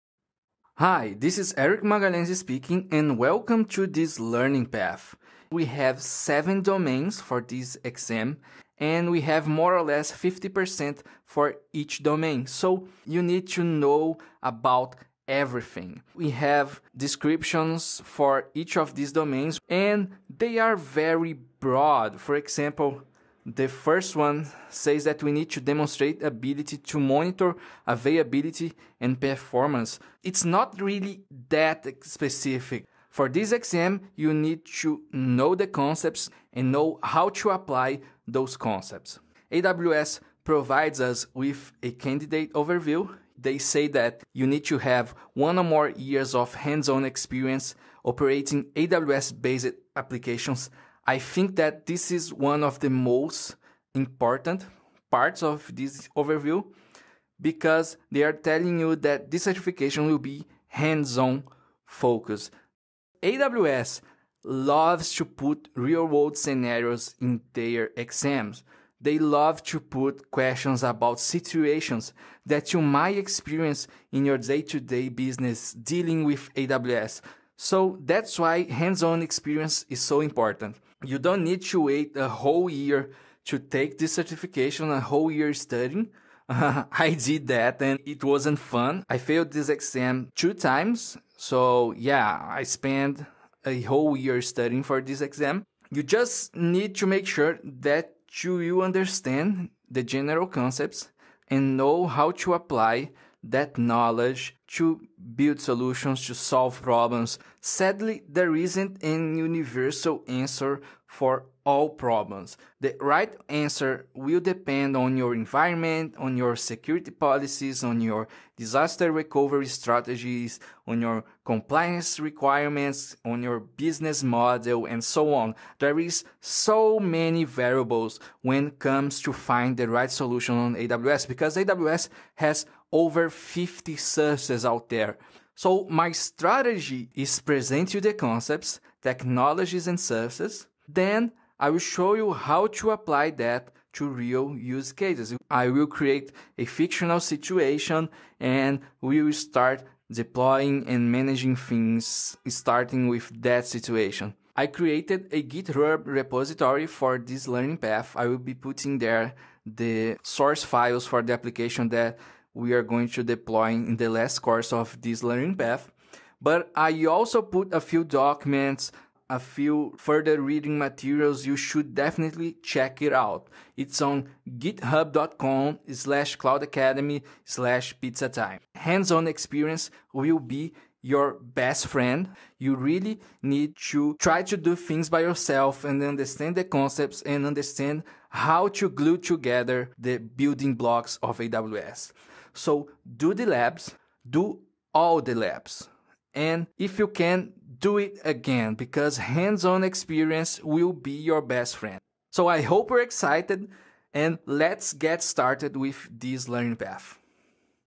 Introduction - SysOps Administrator — Associate Certification for AWS
(Update March 2019) We’ve recently updated the training material of the AWS SysOps Administrator learning path, to prepare for the certification.

Specifically designed to help you prepare for the AWS SysOps Administrator - Associate certification, this learning path is based on the AWS exam guide. This certification demands that the candidate demonstrates the ability to deploy AWS solutions for different scenarios per AWS' best practices, troubleshoot, and tune AWS environments for performance. We will start with a review of the material from the Solutions Architect Learning Path, which will present the foundations for AWS, and in the final course, we will go deep into the AWS console to deploy solutions for common business scenarios.